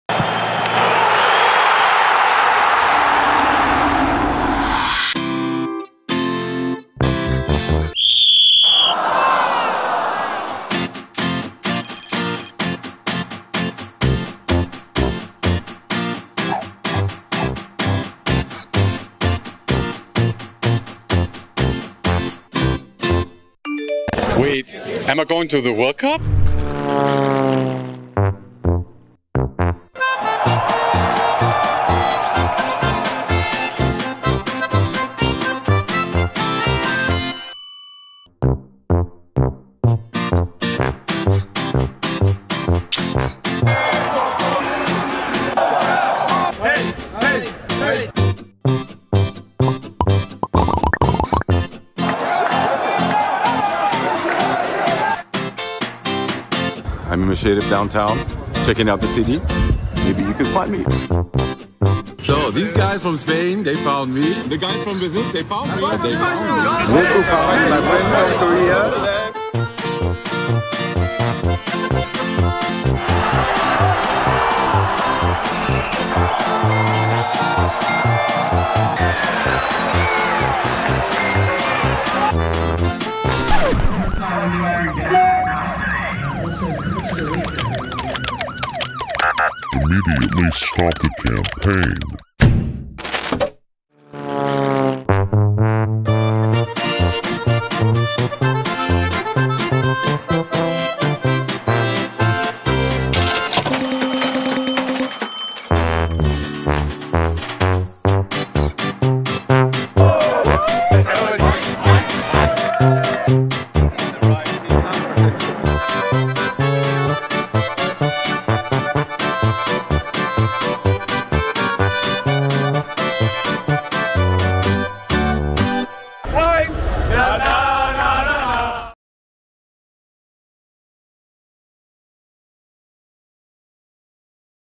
HEINZ: The Heinzjack (Case Study) (Cannes 2023) (Outdoor Bronze)
Outdoor is much more than billboards and posters these days. This case study is a great example from Cannes 2023 that's worth a look.